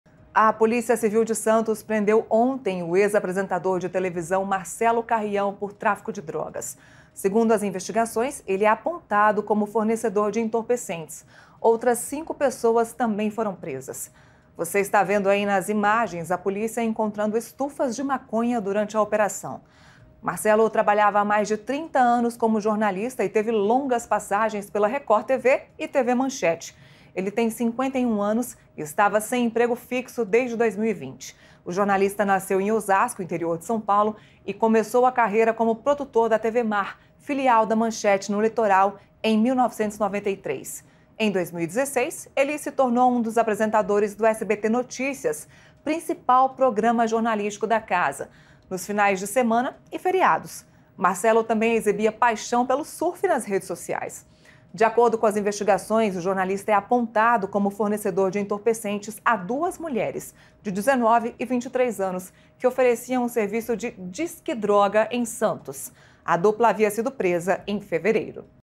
Surfista e repórter policial: quem é ex-apresentador preso por tráf1co
Santos – O ex-apresentador do SBT Marcelo Roberto Ferreira Carrião, preso por tráfico de drogas nessa quarta-feira (28/2) em operação da Polícia Civil de Santos, trabalhava há mais de 30 anos como jornalista e teve longas passagens por TV Record e TV Manchete. O jornalista de 51 anos estava sem emprego fixo desde que deixou o SBT, em janeiro de 2019.